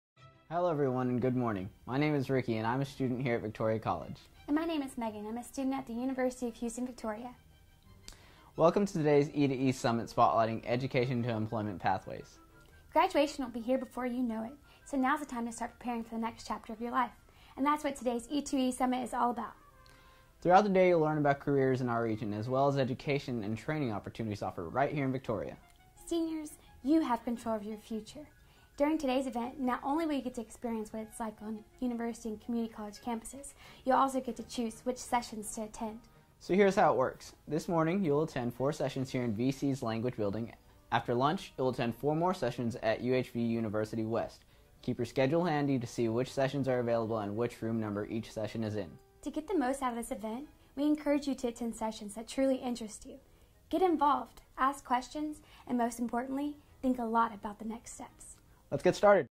E2E Summit - VC
Ricky, a Victoria College student, and Megan, a student at University of Houston-Victoria, give a quick overview of the day's E2E Summit activities.